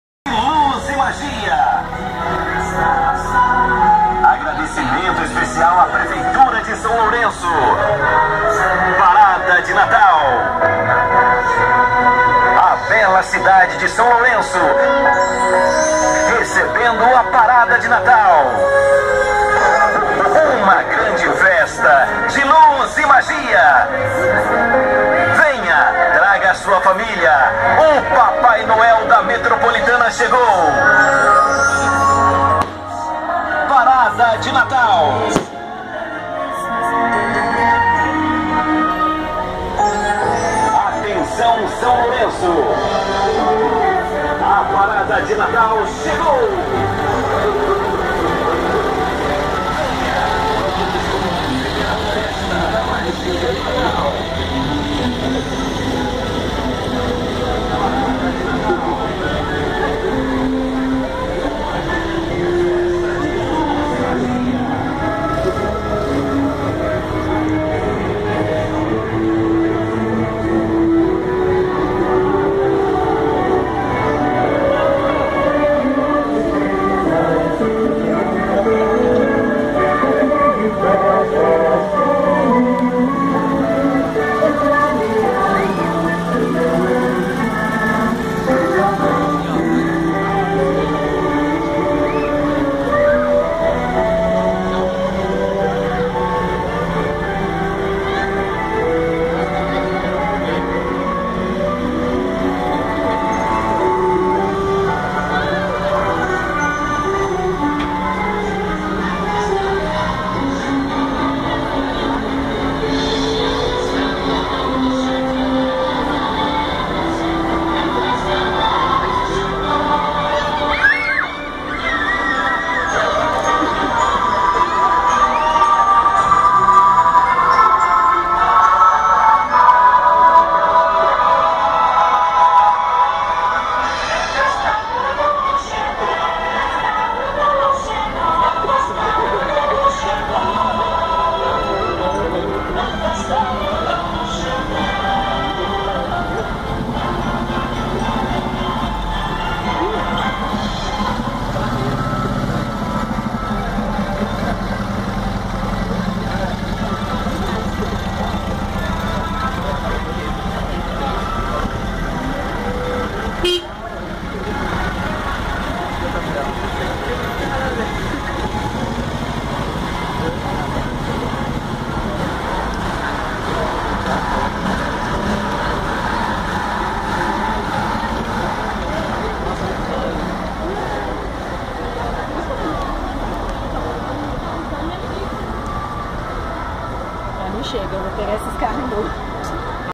PARADA DE NATAL 2014 SÃO LOURENÇO - MG
Mesmo debaixo de chuva, foi grande o número de pessoas que sairam de suas casas e se deslocaram até o centro da cidade de São Lourenço-MG para prestigiar a "Parada Natalina" que percorreu as principais ruas da estância hidromineral.